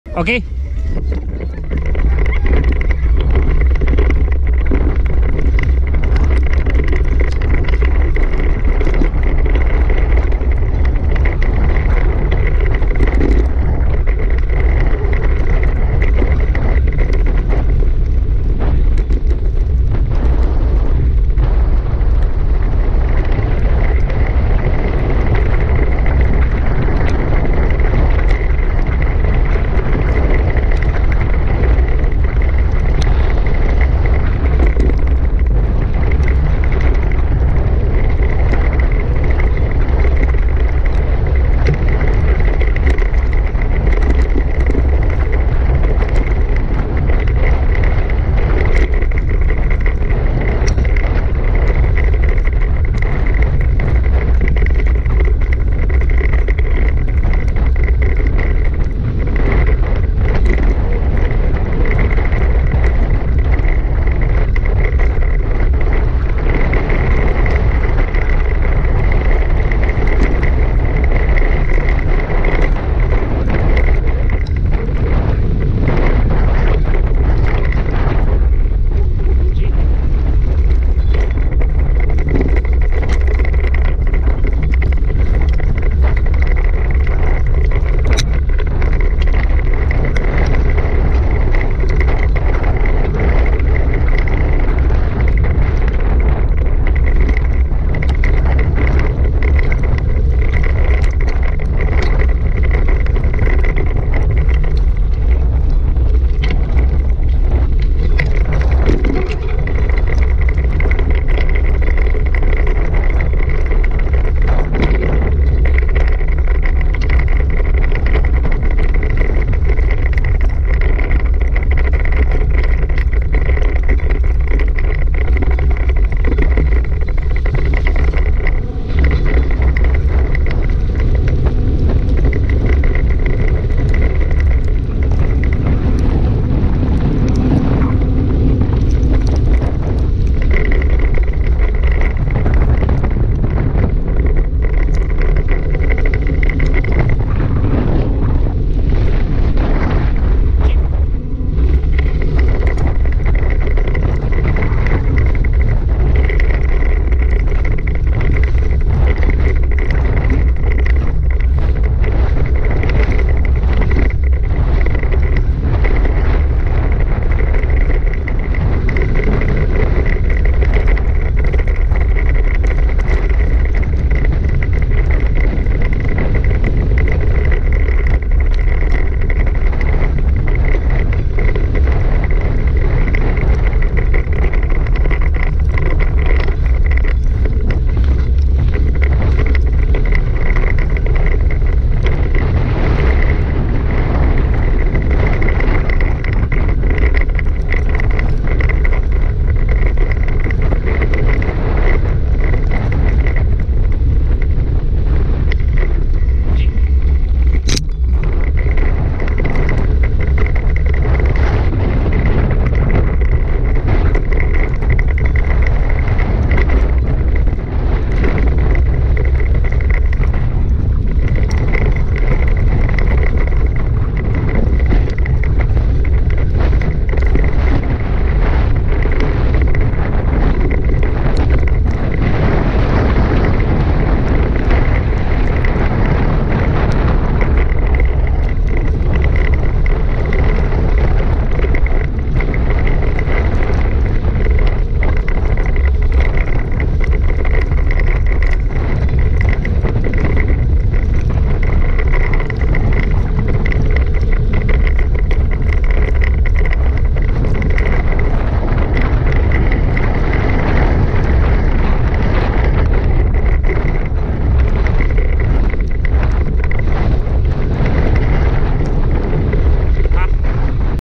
Scooter with Bruno and Harley
Weekend fun and easy run with Bruno and Harley on Bulla scooter. Last dryland training this season.